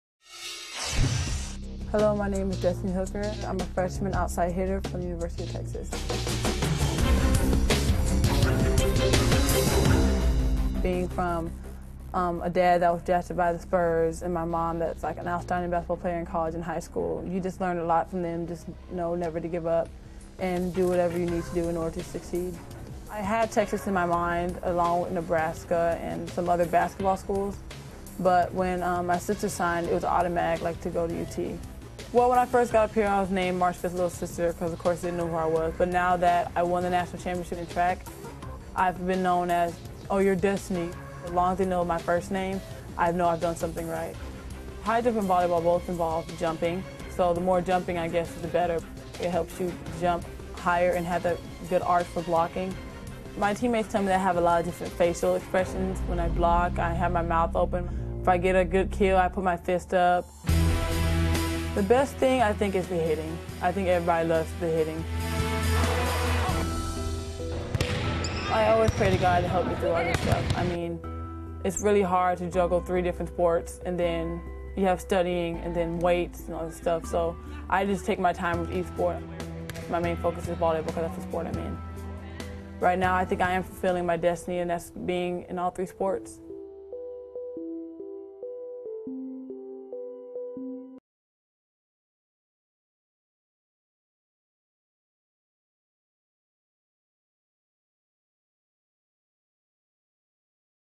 A Star in Texas: Destinee Hooker
The Longhorns star freshman is an all-around great athlete who has taken the game by storm.